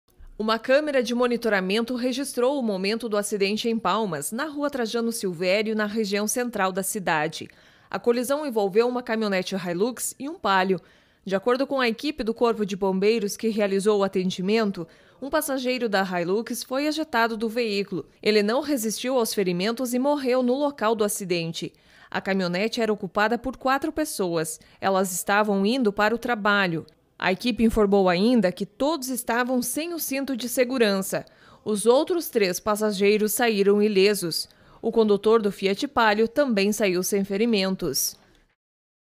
Uma pessoa perde a vida em Palmas-PR após tombamento de veículo
Segurança: Acidente fatal na manhã desta segunda-feira (30/09) em Palmas-PR, deixa uma vítima. Um automóvel tombou após a batida.